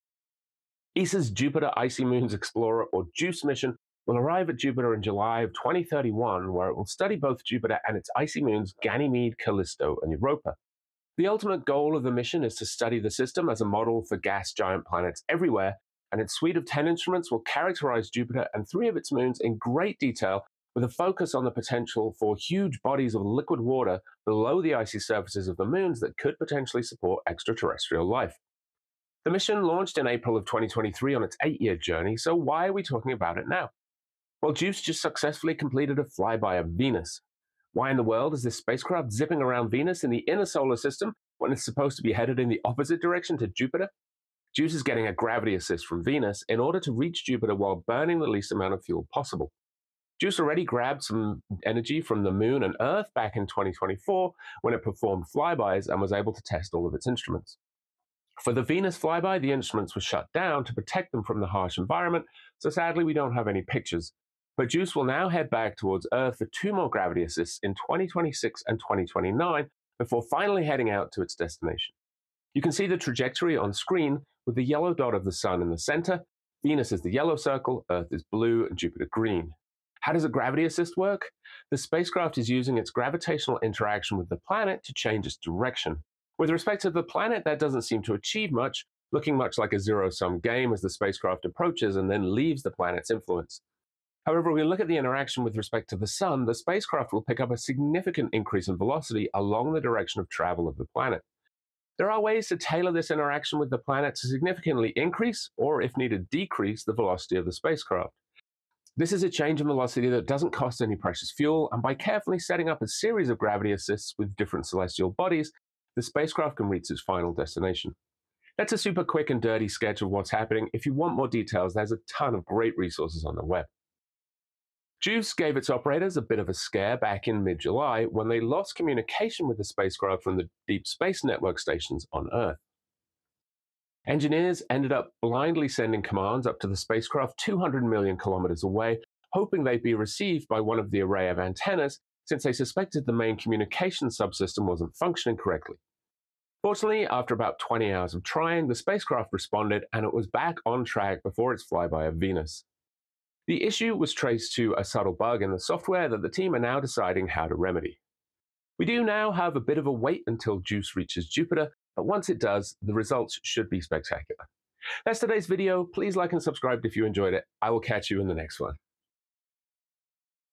ESA's JUICE Zips Past Venus
After briefly losing communications with the spacecraft in July, the JUICE team reports JUICE has just successfully completed a gravity assist maneuver around Venus!  Next stop Earth for 2 more assists before arriving at Jupiter in 2031.